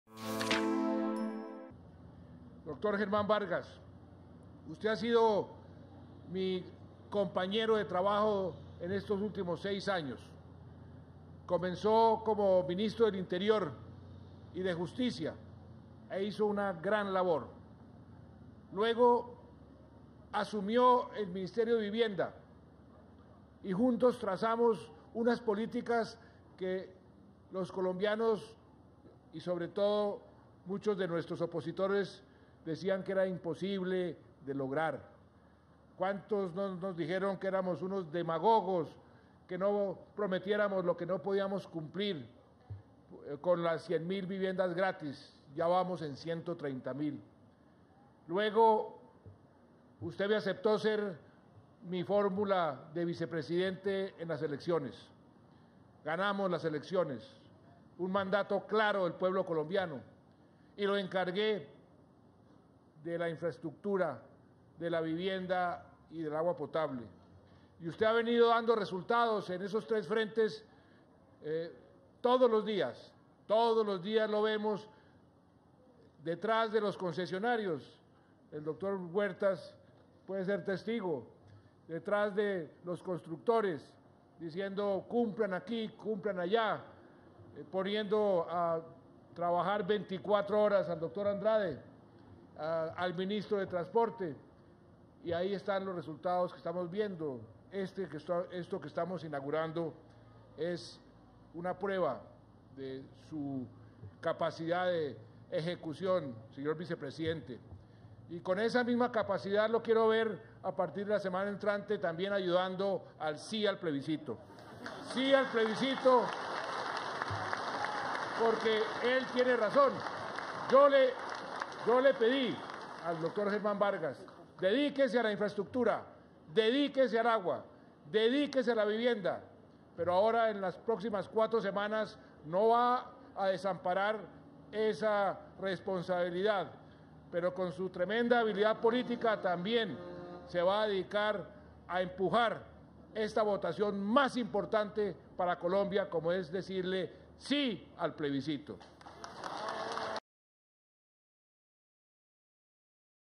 Santos invita a Vargas Lleras a sumarse a la campaña por el 'sí'
El presidente le dijo a Vargas Lleras que lo quiere ver "a partir de la semana entrante también ayudando al 'sí' al plebiscito".